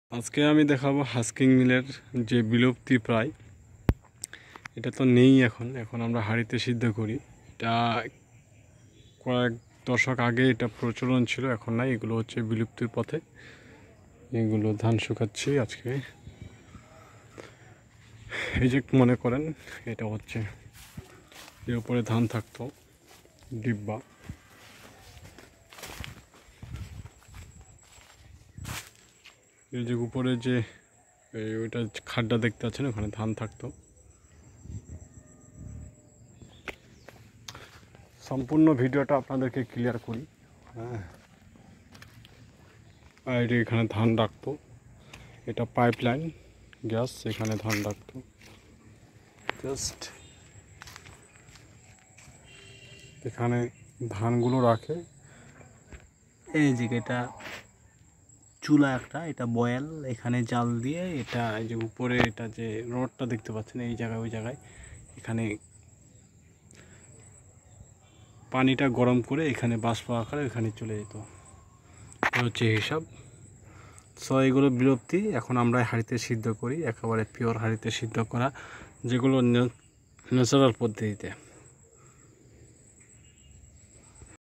হারিয়ে যাওয়া হাসকিং মিলের ধান সিদ্ধ করা ডায়ার আজ নেই বললে চলে।
যুগের সাথে তাল মিলিয়ে চলতে নানান রকমের সমস্যার কারণে আজ হাস্কিং মিল বিলুপ্ত।

এখন আমরাই হাঁড়িতে সিদ্ধ ধানের হাসকিং মিলের চাল দিচ্ছি সারা দেশ জুড়ে সুন্দরবন কুরিয়ার সার্ভিস মাধ্যম।